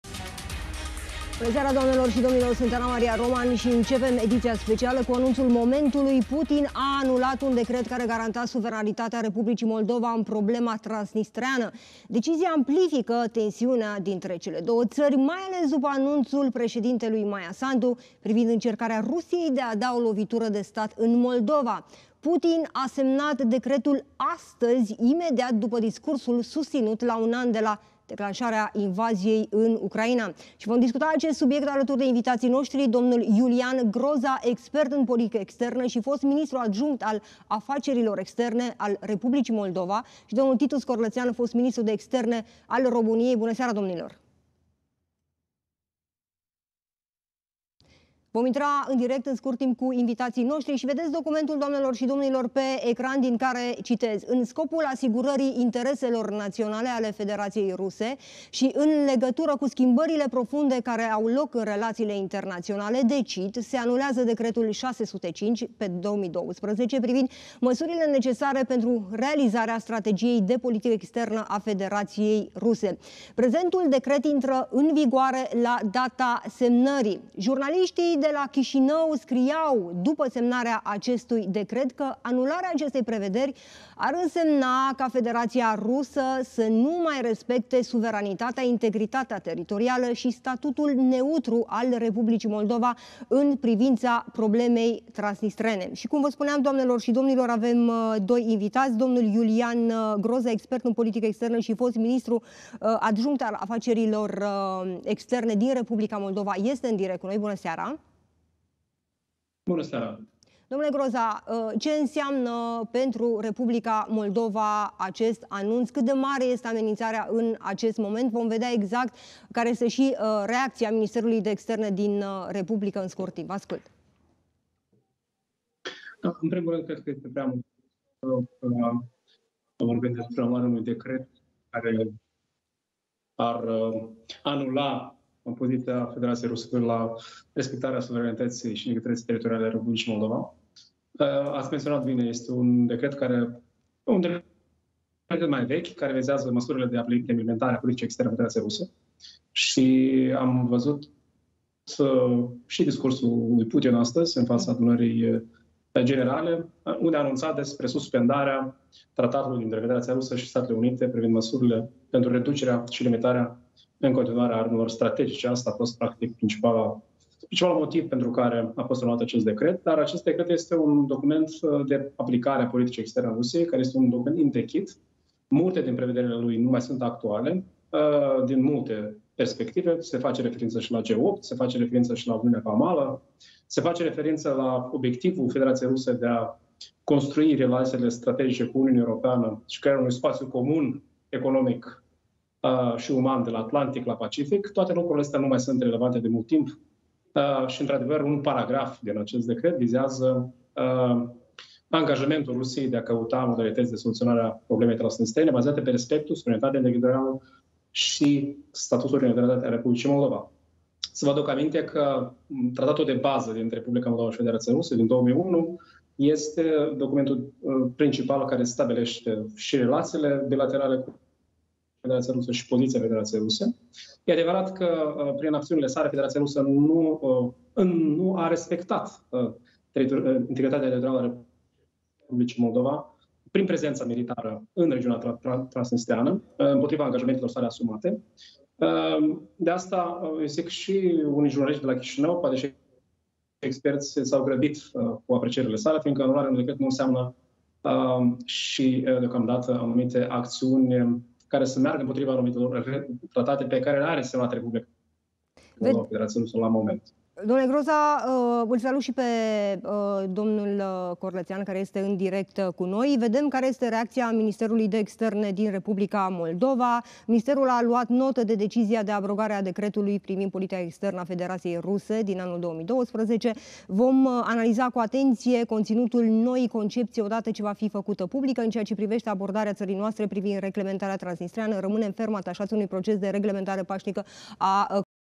Putin a anulat un decret care garanta suveranitatea Republicii Moldova în problema transnistreană
Vladimir Putin a anulat marți, un decret care garanta respectarea suveranității, integrității teritoriale și a neutralității Republicii Moldova în stabilirea statului special al Transnistriei.